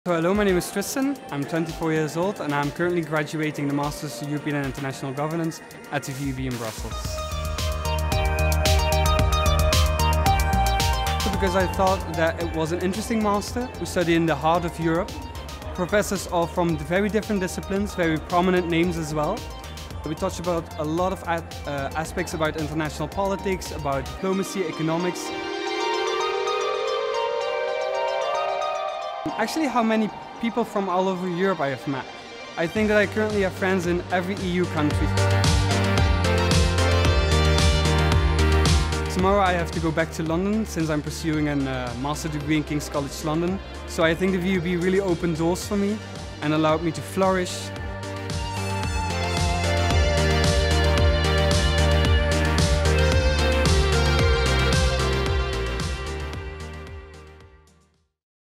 VUB Alumnus Tristan about European & International Governance
Acquire all the skills for a career in diplomacy, (inter)national political relations, and deepen your knowledge on political science in this master's programme

What do you learn in the master's programme?

Keen to acquire the skills and know-how needed for kickstarting a career in diplomacy, national and international institutions or related professional fields (such as lobbying, journalism, policy and academic research)? This is the place!

You'll explore three categories during the curriculum: research methods, skills and leadership development, plus nine mandatory courses. Not forgetting your final master's thesis.

The strengths of our Political Science: European and International Governance master's programme`

- Being in Brussels, you'll be right where it's all happening, with opportunities directly on your doorstep.
- You'll balance academic and theoretical knowledge with current affairs.
- Thanks to our professors involved in the public debate, you'll combine academic knowledge with practical insights and opportunities.